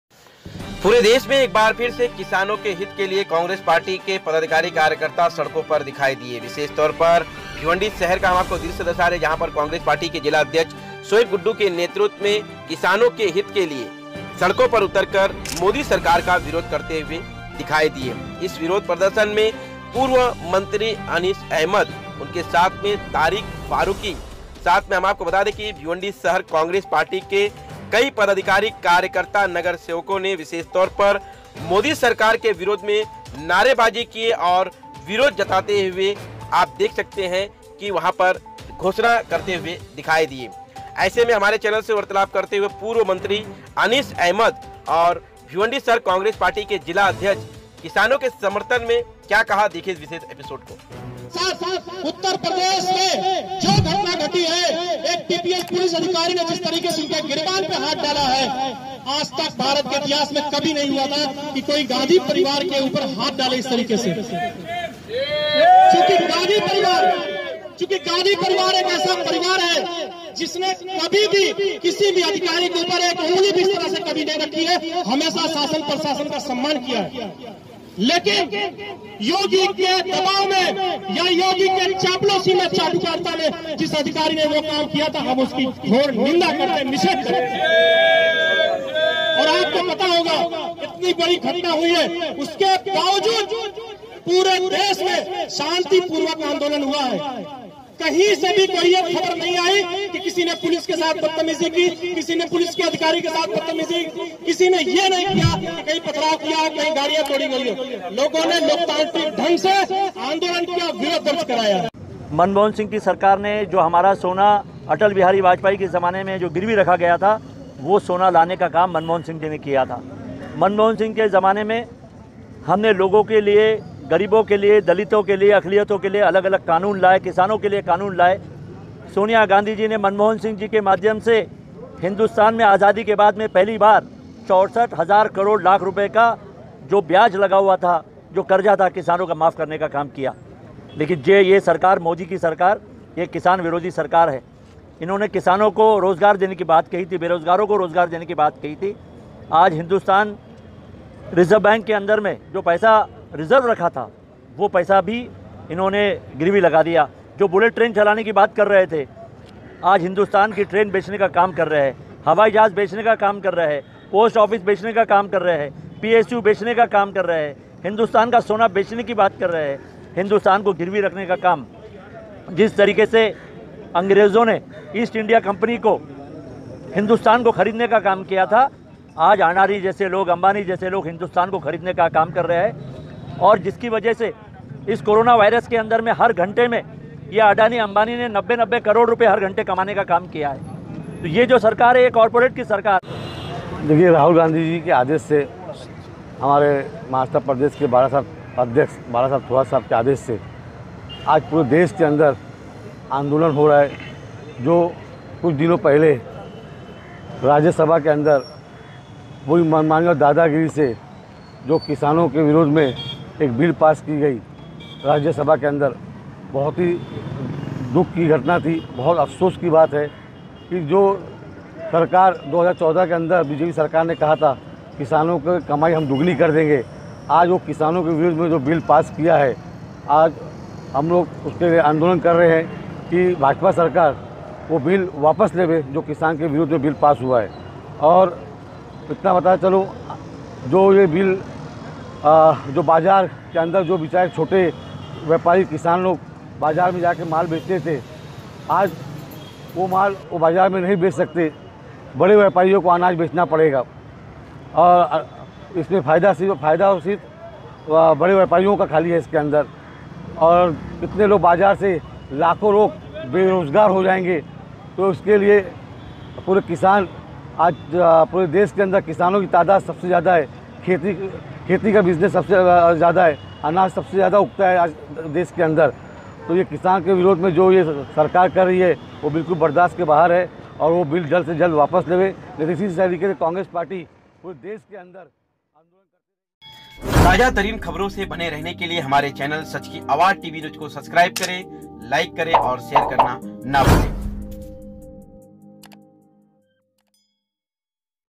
ईस्ट इंडिया कंपनी की तरह भाजपा सरकार हिंदुस्तान को बेचने में लगी है, पूर्व मंत्री अनीस अहमद
पूरे देश में कांग्रेस पार्टी के पदाधिकारी कार्यकर्ता सड़कों पर उतर कर किसान बिल का विरोध करते हुए दिखाई दे रहे हैं भिवंडी शहर कांग्रेस पार्टी के जिला अध्यक्ष श्री गुड्डू के नेतृत्व में पूर्व मंत्री अनीस अहमद की उपस्थिति में भाजपा सरकार का विरोध किया गया क्या कहा विशेष विशेष किशोर को सब्सक्राइब करें लाइक करें शेयर करें हमारा यूट्यूब चैनल सच की आवाज टीवी न्यूज़